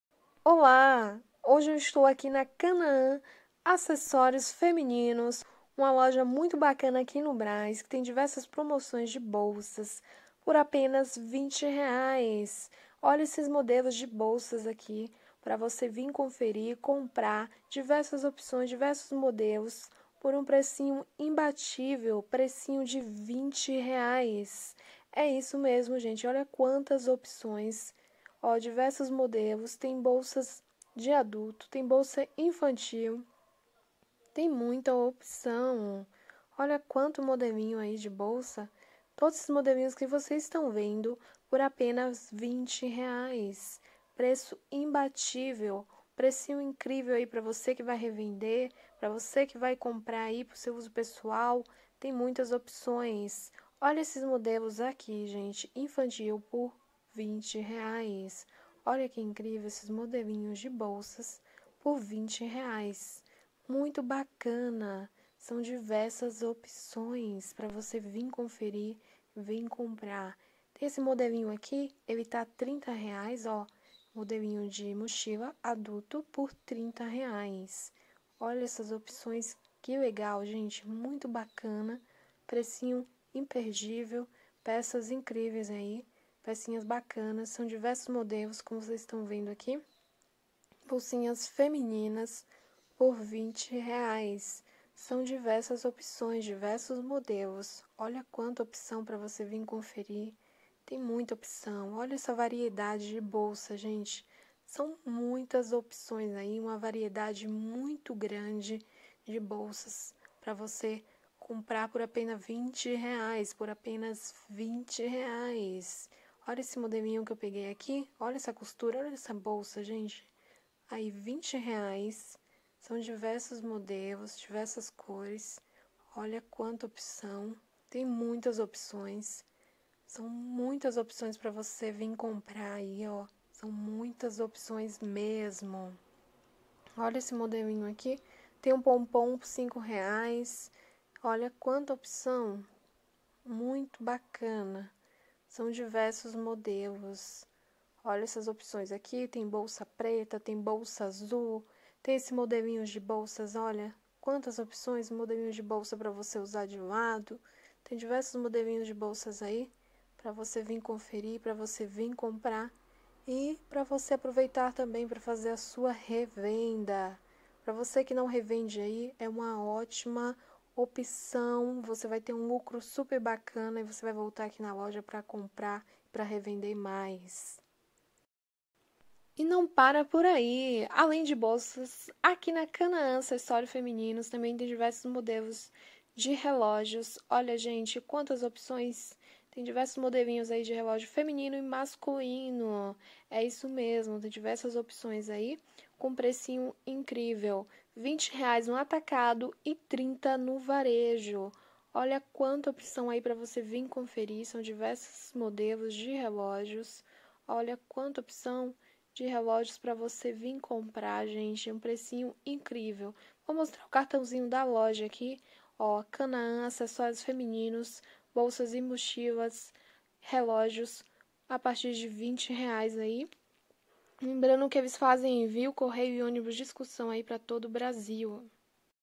Fabricante de bolsas e mochilas no Brás
Bolsas de 20,00 reais no Brás
Endereço: Rua Rangel Pestana, 1899 dentro da galeria  Torres Brás, próximo ao metrô Brás.